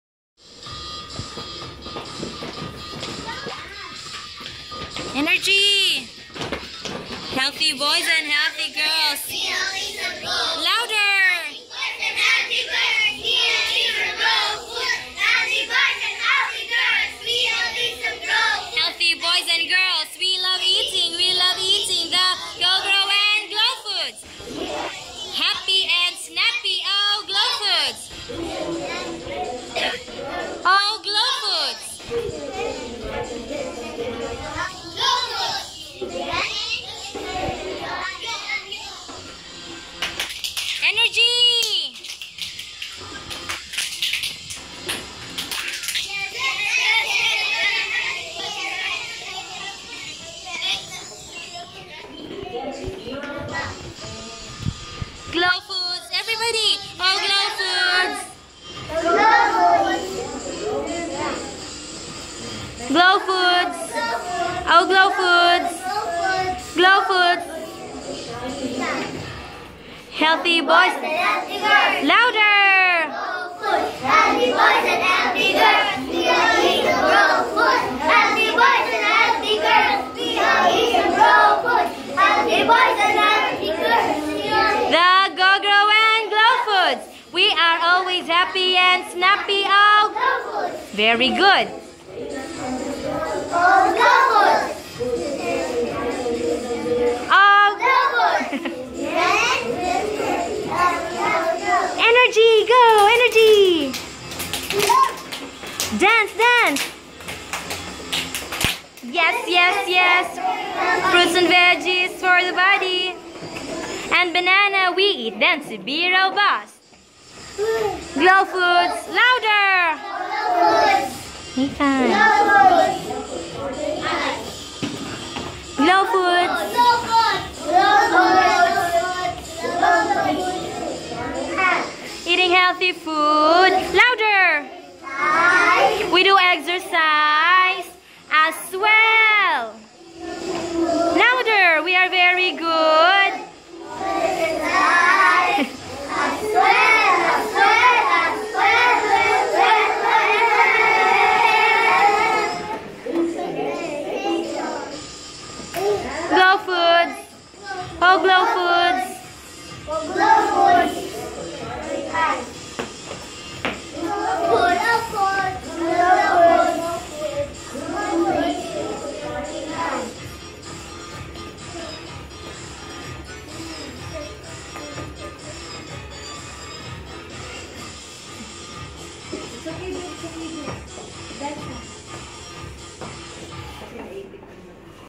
PSD Kinder 2- Alert, Nutrition Month Celebration
Azza and classmates celebrating nutrition month, Kinder 2 - Alert, Philipine School Doha, Qatar.